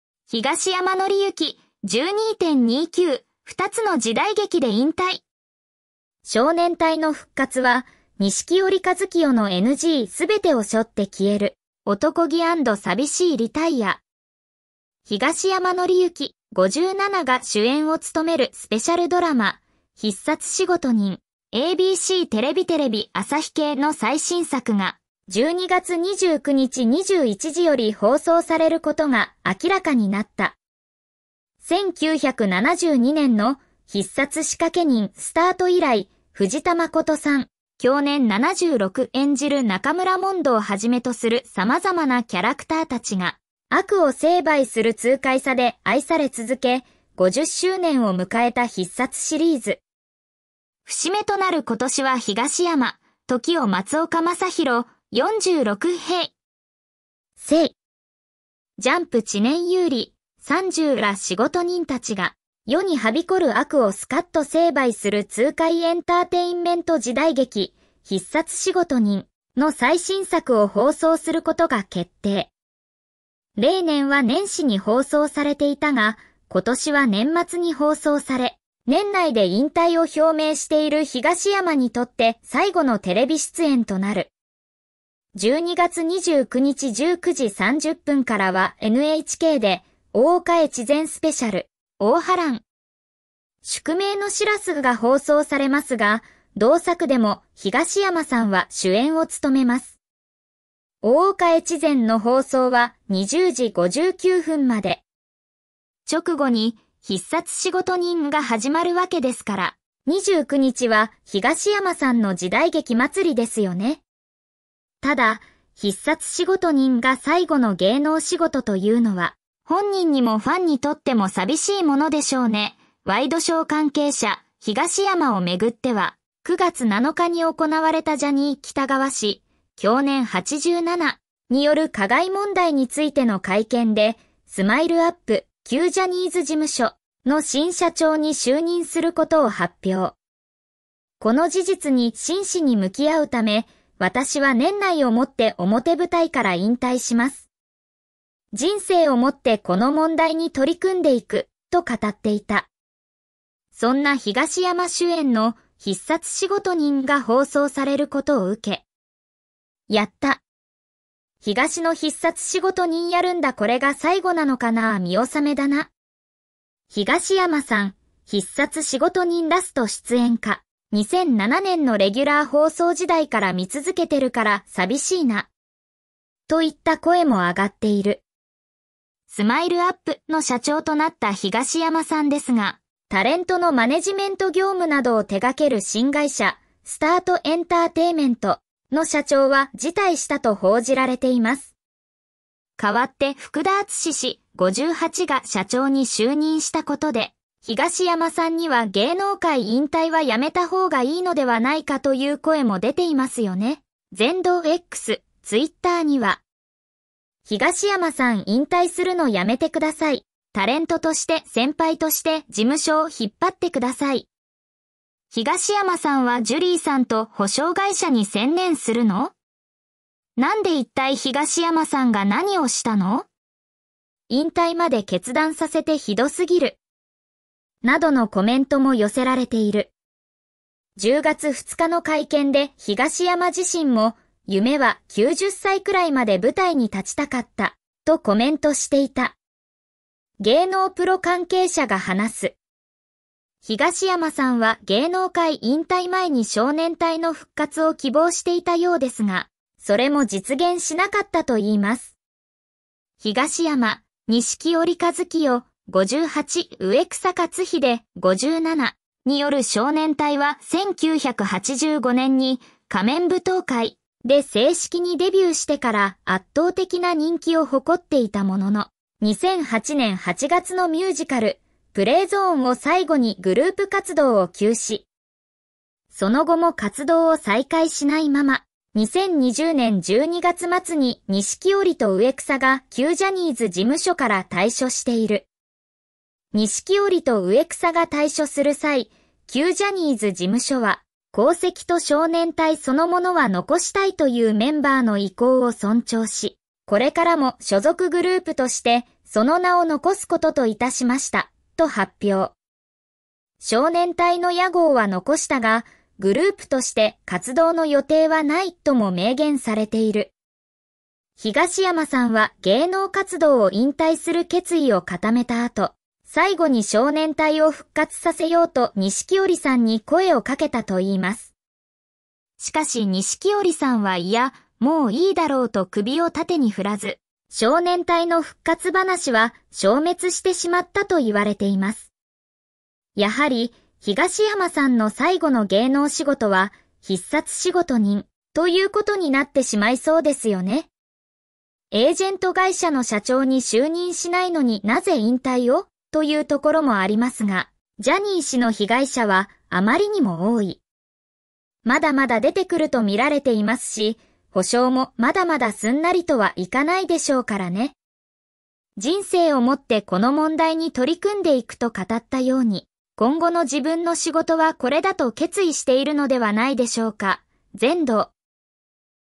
東山紀之、2つの時代劇で芸能界に別れを告げる！少年隊の復活はならず…「男気＆寂しいリタイア」
ジャニーズ事務所の社長に就任した東山紀之が、2023年末をもって芸能活動を引退することを発表した。引退前に出演するのは、NHK大河ドラマ『源氏物語』とテレビ朝日系ドラマスペシャル『大岡越前』の2つの時代劇だ。東山はこれまでに数々の時代劇で主演を務め、その演技力と存在感で視聴者を魅了してきた。しかし、かつての仲間である錦織一清と植草克秀との再結成は実現せず、少年隊の復活の夢は潰えた。東山はすべてを背負って消える「男気＆寂しいリタイア」を選んだのだろうか。